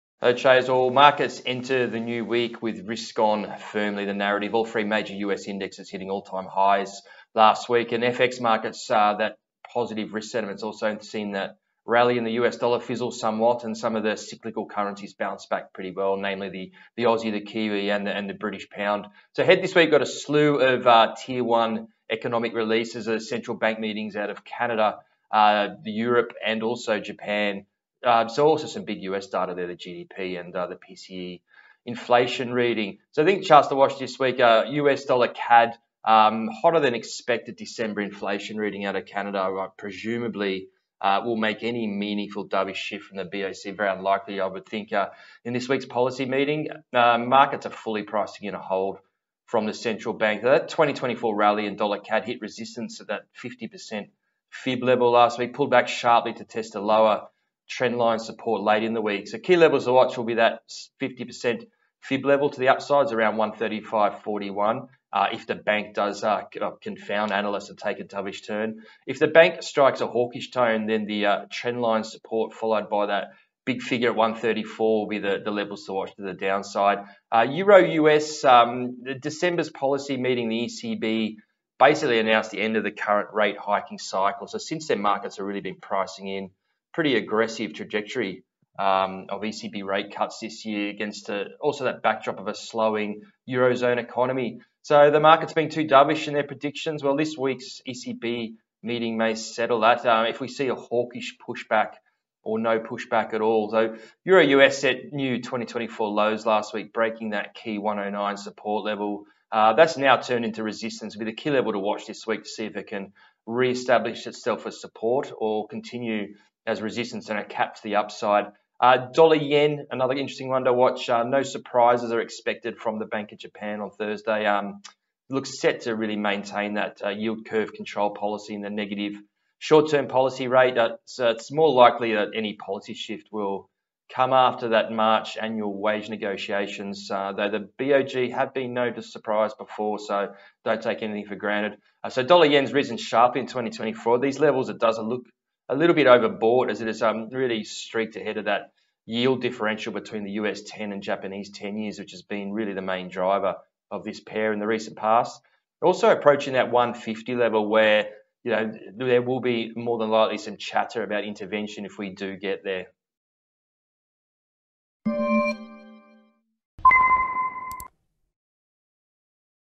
The Week Ahead - BoJ, ECB, BoC
Ahead this week, traders have a slew of risk events to navigate with Central Bank meetings in Canada, Japan, and Europe set to headline, also some big US data in Q4 GDP and the PCE inflation reading set to move FX markets. Charts to watch: USDCAD, USDJPY, EURUSD